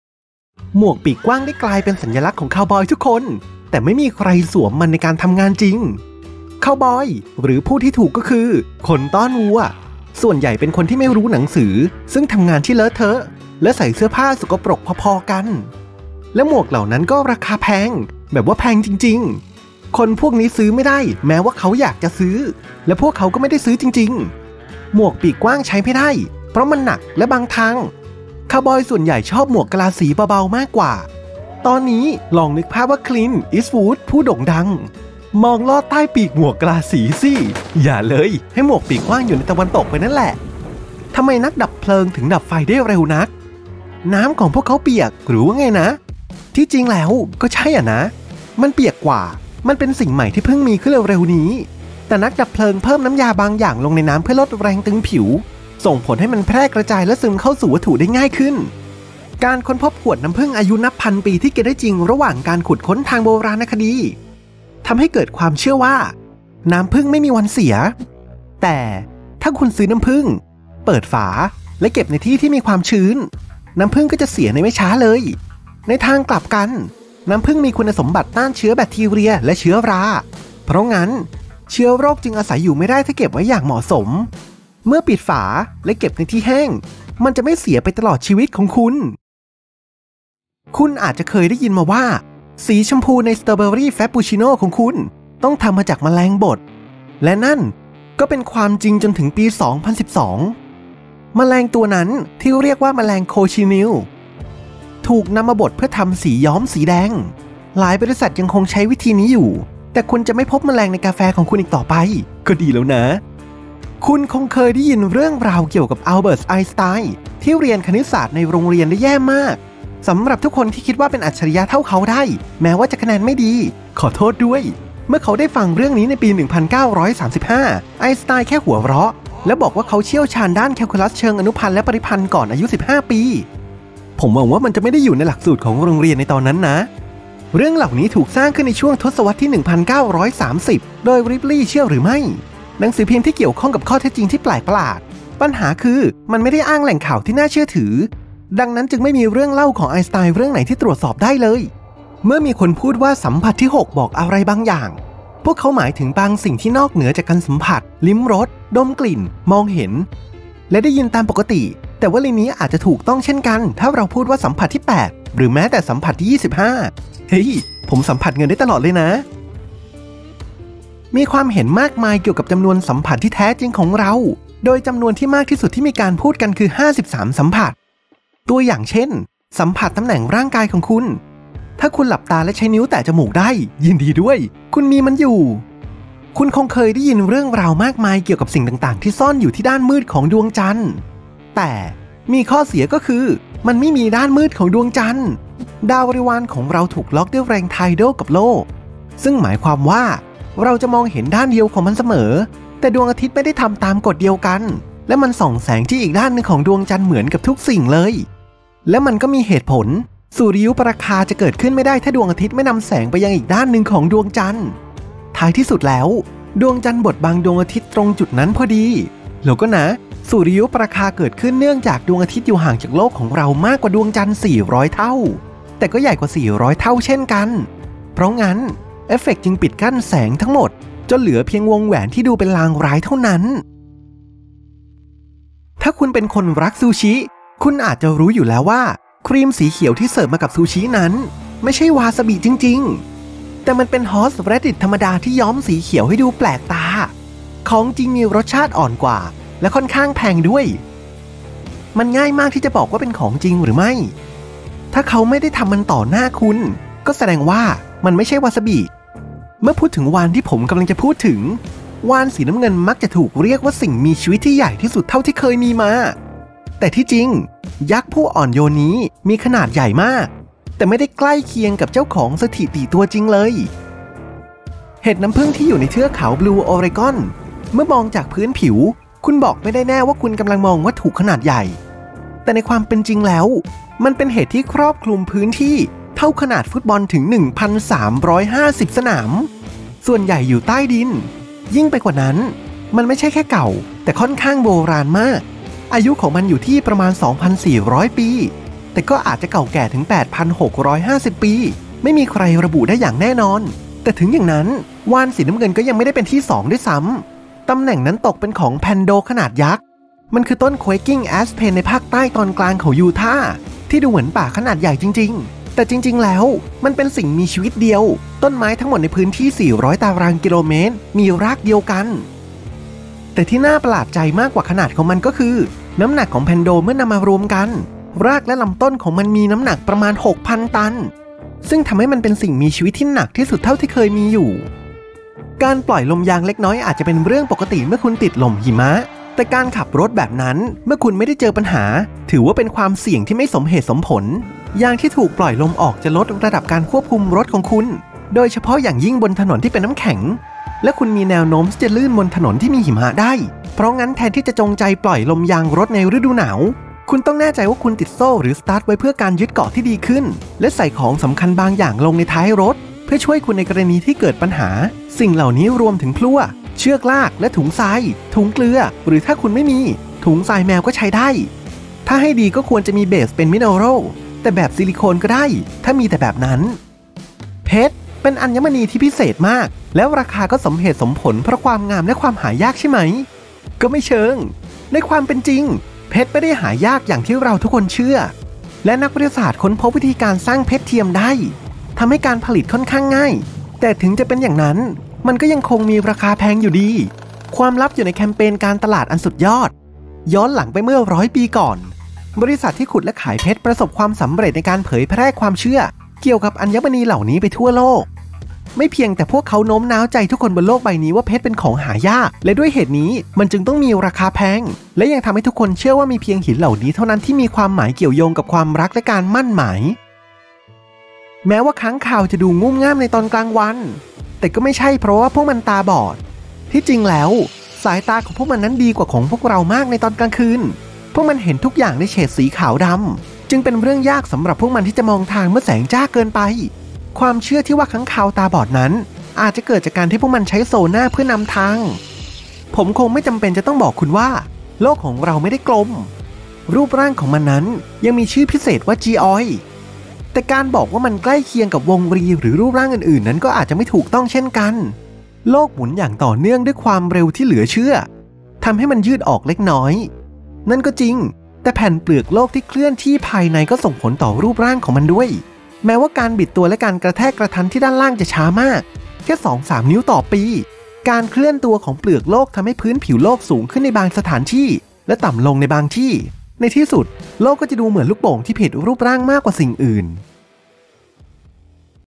ความเชื่อที่ฉันคิดว่าเป็นเรื่องจริงมาทั้งชีวิต
คุณเคยเชื่ออะไรบางอย่างแล้วพบว่ามันเป็นไม่จริงหรือเปล่า มีข้อเท็จจริงมากมายในโลกนี้ที่เราคิดว่าเป็นจริงแต่แล้วกลับกลายเป็นเรื่องโกหก!

ตัวอย่างเช่น คุณรู้หรือมั้ยโลกของเราไม่ได้กลม รูปร่างของมันยังมีชื่อพิเศษด้วย จีออยด์ ฉันจะแสดงให้คุณเห็นว่ามันหน้าตาเป็นยังไงแต่มีสิ่งหนึ่งที่แน่นอนคือโลกหมุนด้วยความเร็วที่เหลือเชื่อ ทำให้มันยืดออกเล็กน้อย ก็จริง แต่แผ่นเปลือกโลกที่เคลื่อนที่ภายในก็ส่งผลต่อรูปร่างของมันด้วย